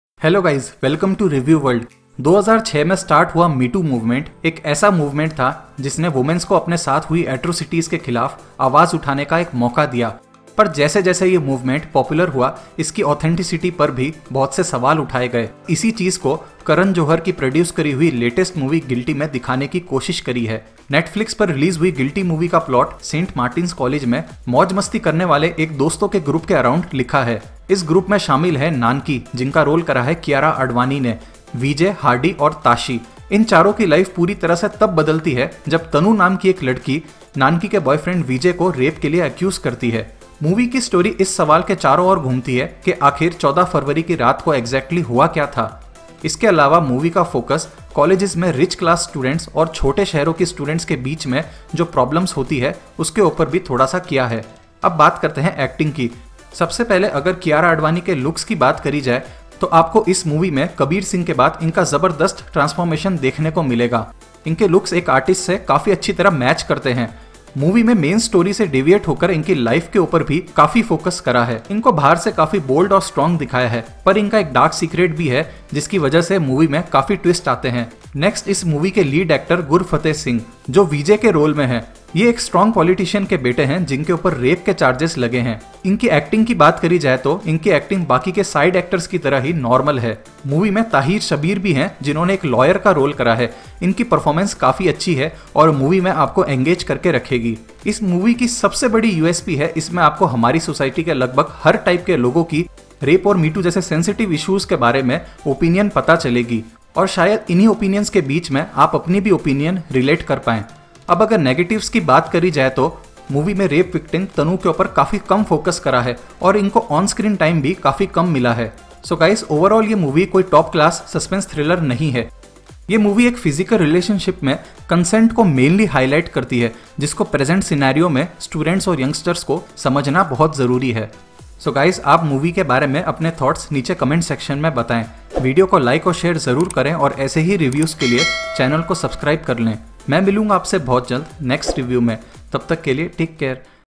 GUILTY NETFLIX Movie Review | Kiara Advani, Karan Johar
Guilty Movie Review | Netflix's Guilty 2020 Movie Full Review | Guilty Spoiler free Review

Guilty is a netflix indian film directed by Ruchi Narain and written by Narain, Kanika Dhillon and Atika Chohan. Starring Kiara Advani, gurfateh singh, taher shabbir and several others, the film follows the story of nanki whose boyfriend Vj is accused of rape. The film is the first production venture of Dharmatic, the digital arm of Karan Johar's Dharma Productions. It was released on 6 March, 2020 on Netflix.